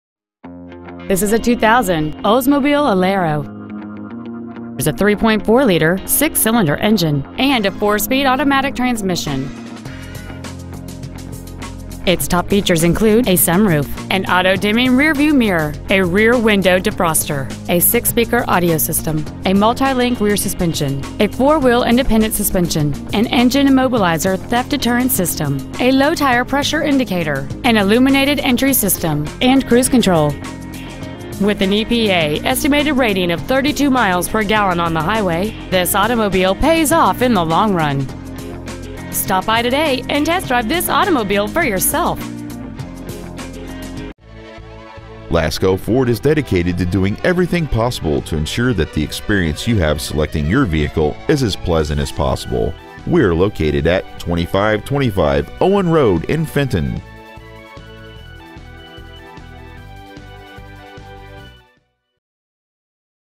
2000 OLDSMOBILE ALERO Fenton MI
We are proud to present this 2000 OLDSMOBILE ALERO .

 Year : 2000

 Make : OLDSMOBILE

 Model : ALERO

 Engine : 3.4L V6

 Trans . : AUTO 4SPD

 Exterior : BLUE

 Miles : 184,516

 Lasco Ford

 2525 Owen Road